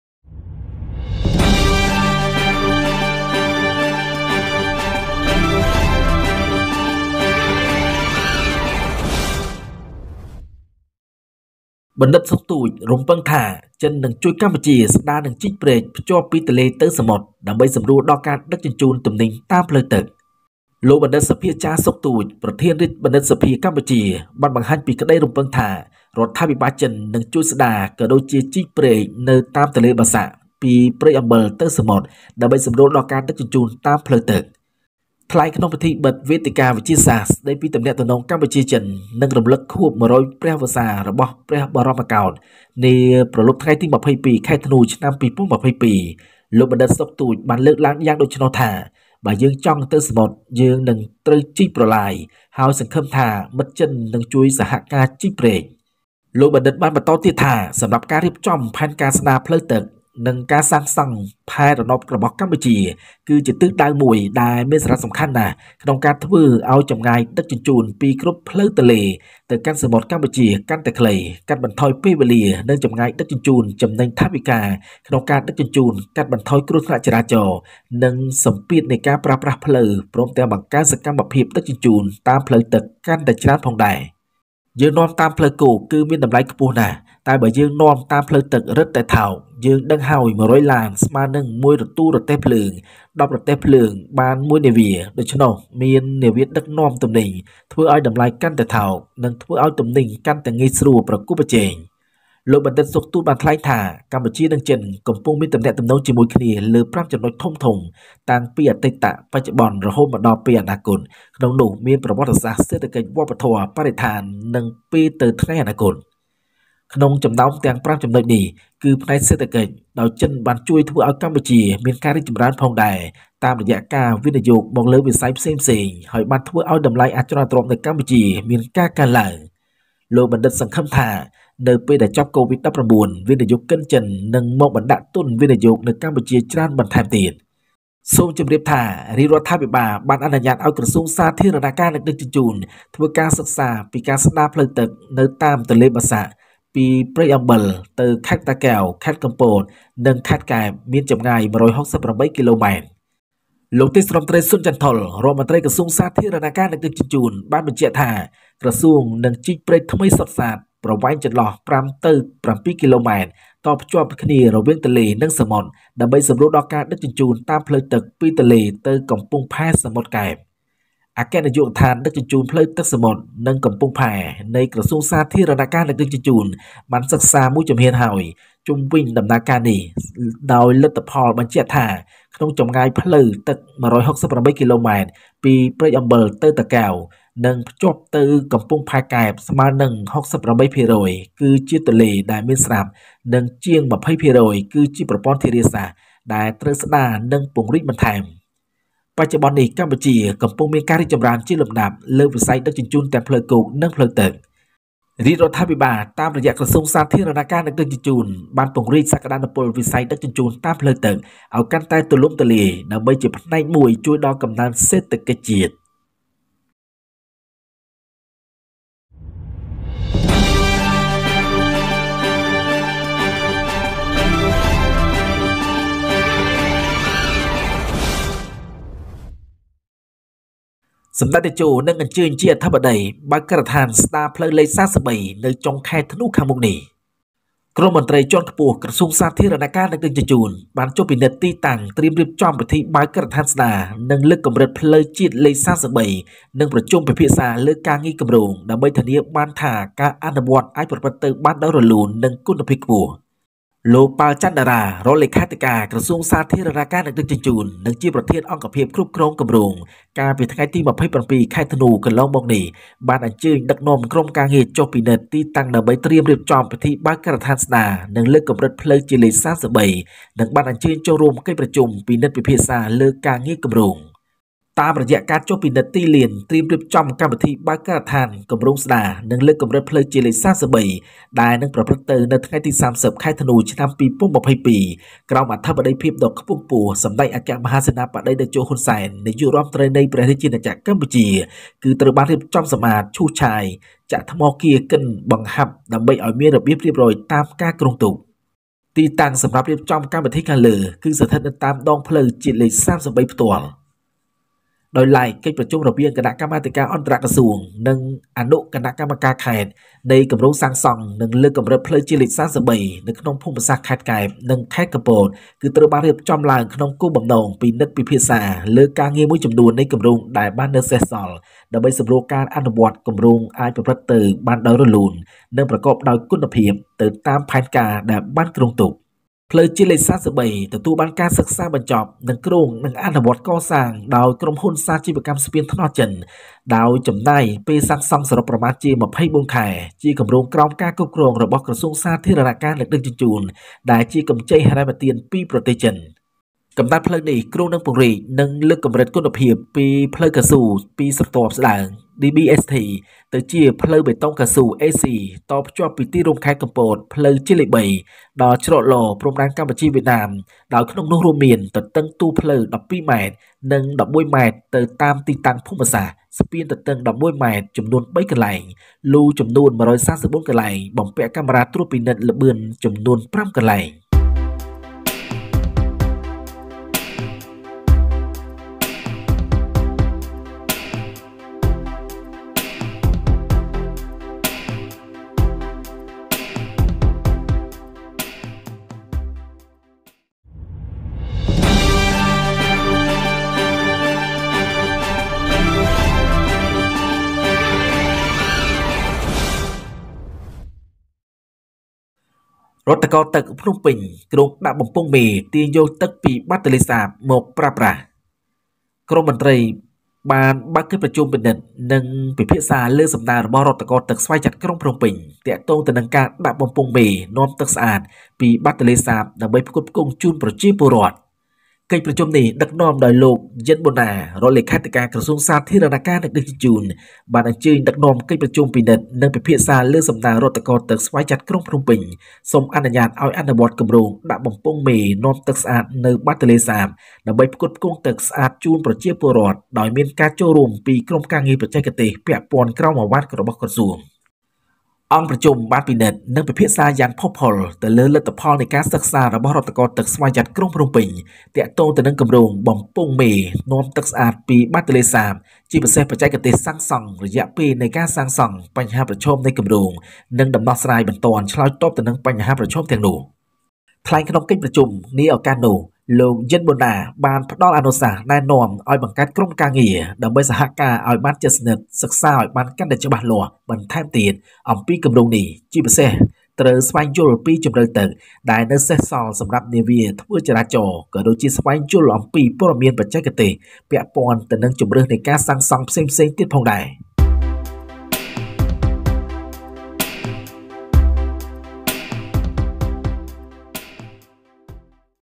កម្ពុជារំពឹងថាចិននឹងជួយកម្ពុជាស្តារនិងជីកព្រែកភ្ជាប់ពីទន្លេទៅសមុទ្រ
Dr. Sok Touch, President of the Royal Academy of Cambodia, expressed hope that the Chinese government would help restore and dig canals along the Bassac River from Prek Ambel to the sea to facilitate water transport. Speaking at the closing ceremony of the scientific forum on "Cambodia-China relations and commemoration of the 100th anniversary of His Majesty the King" on the evening of December 22, 2022, Dr. Sok Touch said: "If we want to go to sea, we must dig  The canal and hope that Chinese friends will help dig the canal. Dr. continued that for the planning of the restoration of waterways and the construction of the satellite of Cambodia is a very important direction to shorten the transport distance from all rivers to the sea, reduce the time and distance.Transportation saves on transportation costs, reduces traffic accidents and road congestion pressures, as well as increases waterway transport activities. "We take it by land, it is very expensive, but if we take it by water it is cheaper. We know that 100 cars is equal to one train, 10 trains, one train, so having a cargo ship makes the price cheaper and it will be easier to carry goods."  Compete. ”

 Dr. Sok Touch said that Cambodia and China are related on five major points from the past to the present, including history, economy, culture, environment and the future.

 Among these five points is the economic sector, with China helping to make Cambodia prosperous by investing in other sectors and increasing property prices in Cambodia.  He hopes that at the end of Kovid-19, Chinese investors will come to invest more in Cambodia.

 Please be informed that the Royal Government has authorized the Ministry of Public Works and Transport to study the restoration of waterways along the Bassac River from Prek Ambel to Takeo, Kampot and Kep provinces, a distance of 168 kilometers.

 Senior Minister Sun Chanthol, Minister of Public Works and Transport, confirmed that the Ministry will dig a new canal between 5-7 km between the river and the sea to facilitate water transport from the river to the port of Kep.

 The General Department of Inland Waterway Transport and Ports of the Ministry of Public Works and Transport have taken a step-by-step study of this process, with the results stating that the 168-kilometer waterway from Prek Ambel to Takeo and connected to Kep Port is 68%.  Existing rivers and more than 20% are irrigation systems that need to be rehabilitated and expanded.

 Currently, Cambodia is making steady progress in the field of transportation, both land and water.  The Royal Government, through the Ministry of Public Works and Transport, has expanded the potential of the water transport sector to be part of the national economic growth.